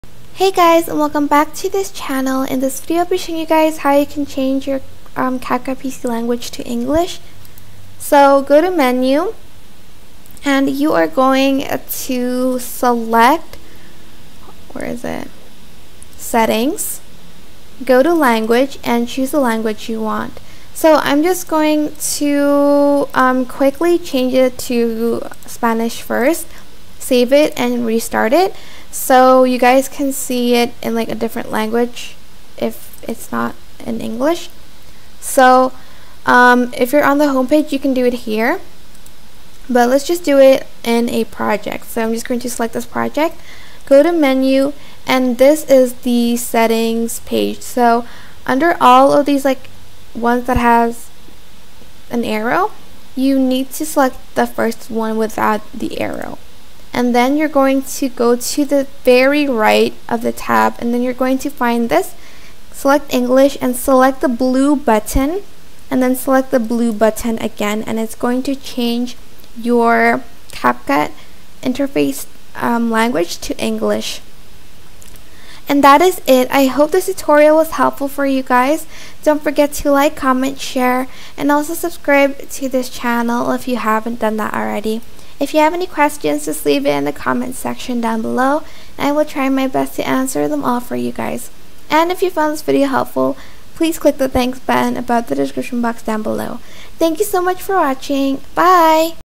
How To Change CapCut PC Language To English
Want to change the language on your CapCut PC app? Here's how you can do that.